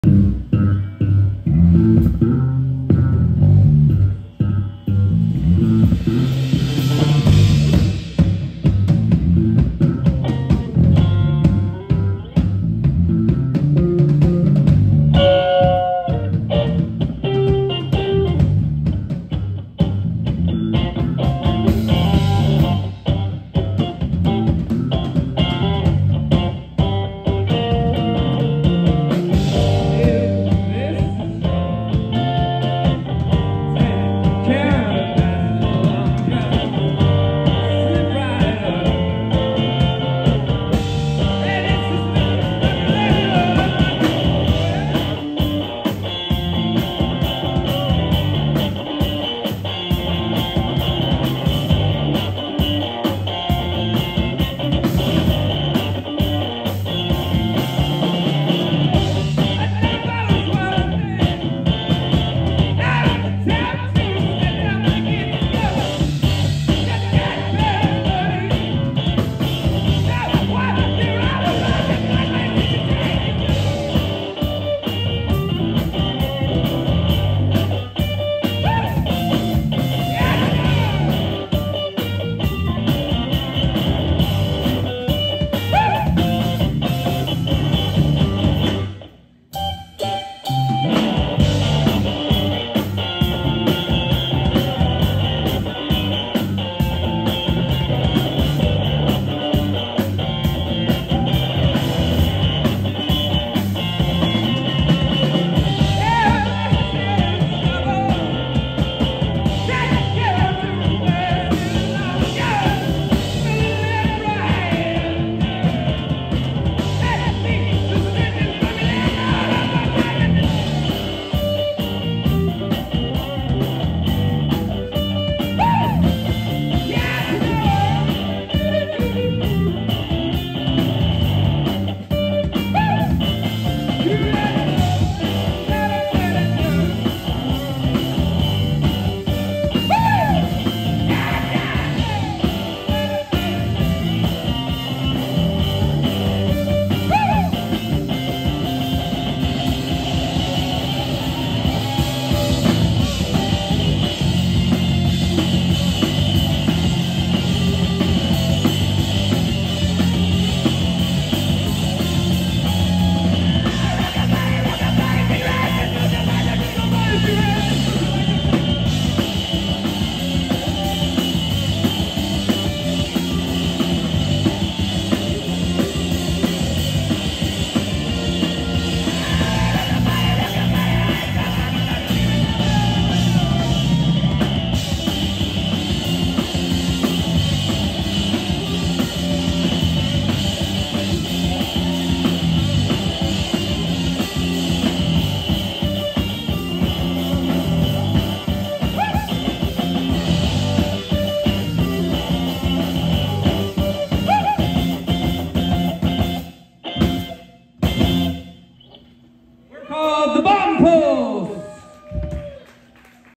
The Bomb Pulse (5/26/24)
The Bomb Pulse playing The Fred Hampton Free Store in New Orleans, LA. 5/26/24